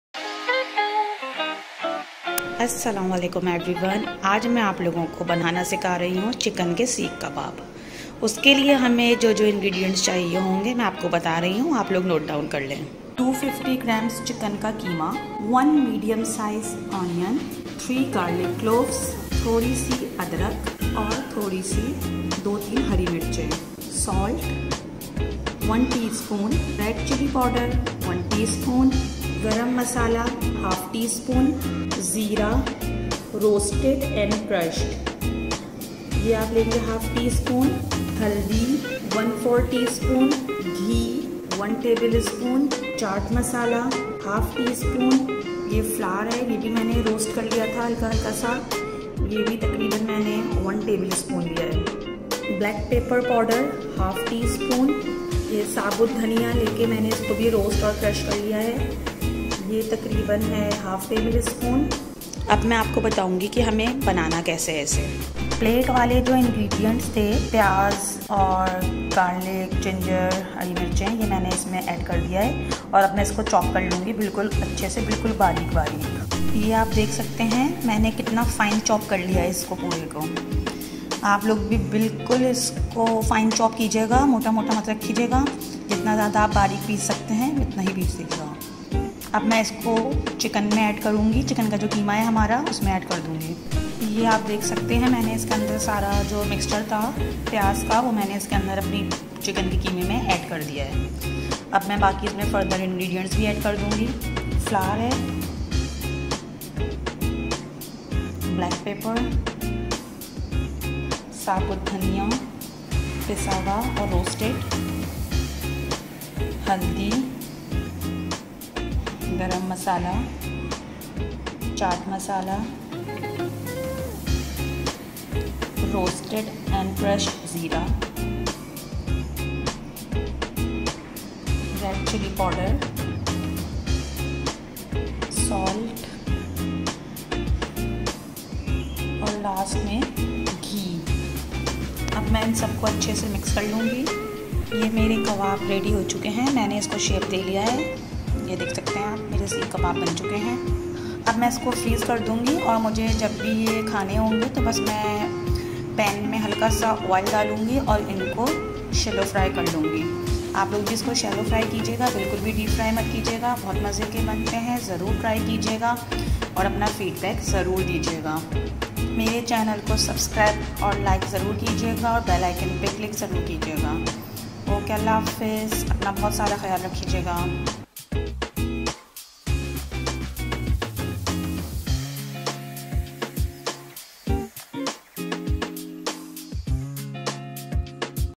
Chicken Seekh Kebab
Kebabs are famous all over the world. These are not only delicious but easy to eat. So, what are you waiting for? Get your hands on the ingredients & start cooking this mouth watering chicken seekh kebab recipe & serve them with hot parathas & green chutney!! :)

Recipe

Chicken                                                        250gm
Onion                                                            1 (medium)
Garlic Cloves                                                3pc
Ginger                                                           As per Taste
Green Chilies                                               5 to 6
Salt                                                               1te.sp
Red chili powder                                        1te.sp
Spice Powder                                              ½te.sp
Cumin seeds (roasted & crushed)           ½te.sp
Turmeric powder                                        ¼te.sp
Ghee                                                             1tb.sp
Chat masala                                                ½te.sp
Flour (Roasted)                                           1tb.sp
Black pepper Powder                                 ½te.sp
Coriander (roasted & crushed)                 ½ tb.sp